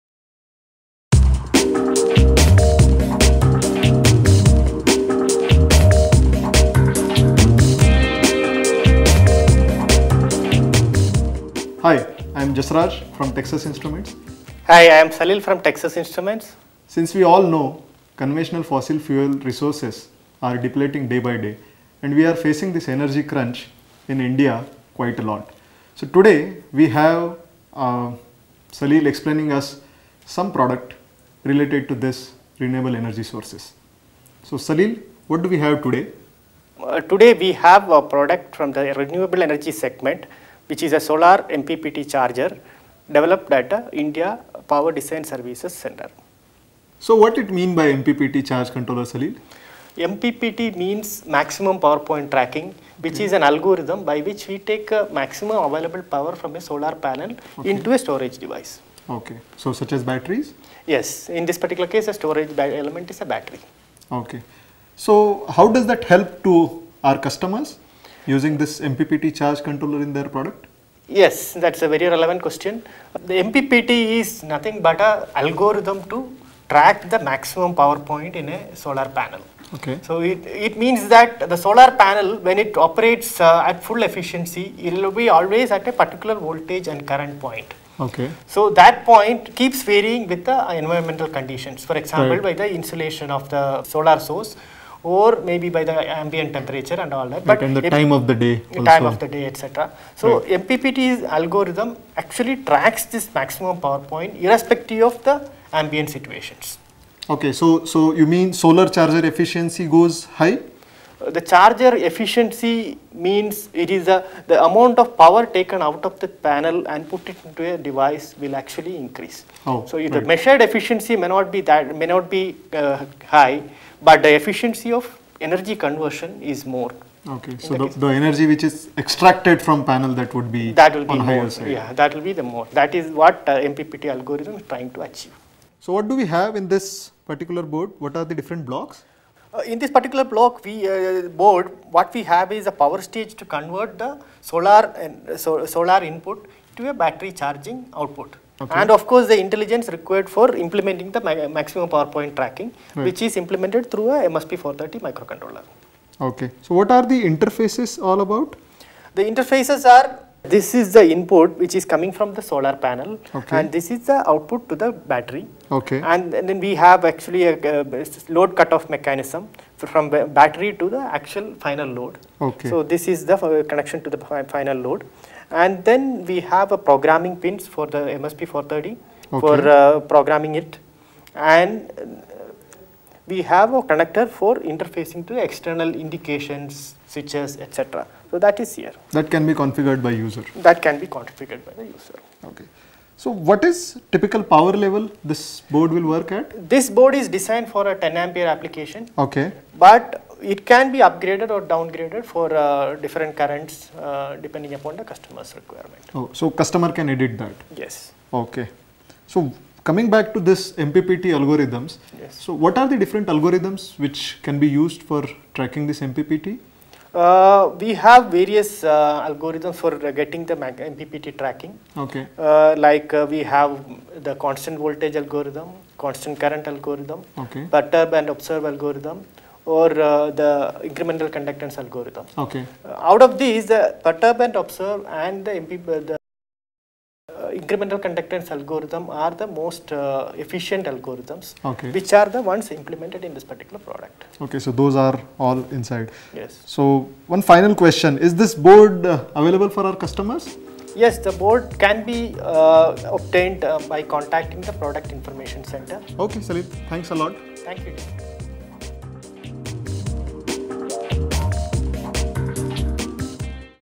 MPPT Solar Charger
Jasraj Dalvi, a Systems Application Engineer from TI India and Salil Chellappan, Lead Engineer for TI's Power Design Services team in TI India disscus an MPPT solar charger reference design.